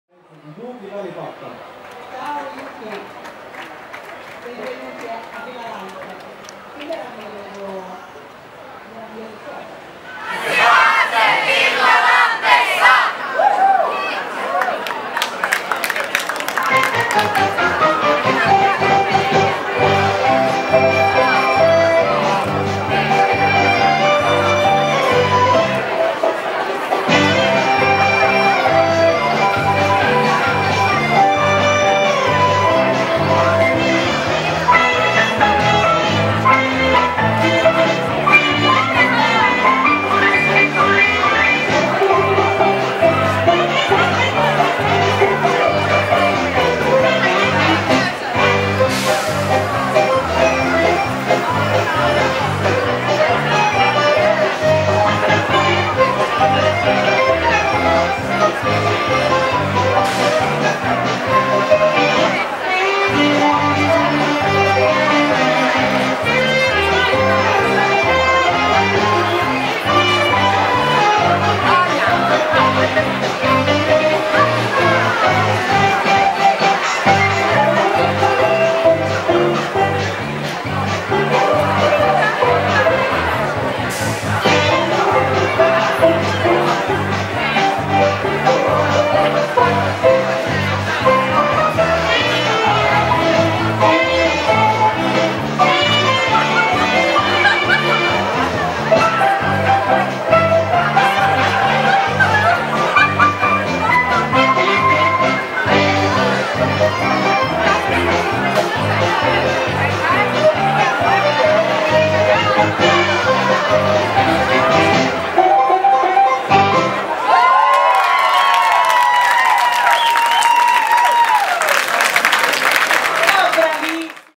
Villa Lante Tanssihaaste
Rooman Villa Lantessa osallistuttiin tanssihaasteeseen 100-vuotiaan Suomen ja lähestyvän juhannuksen kunniaksi! Tapahtuman järjesti Suomen Rooman Instituutti.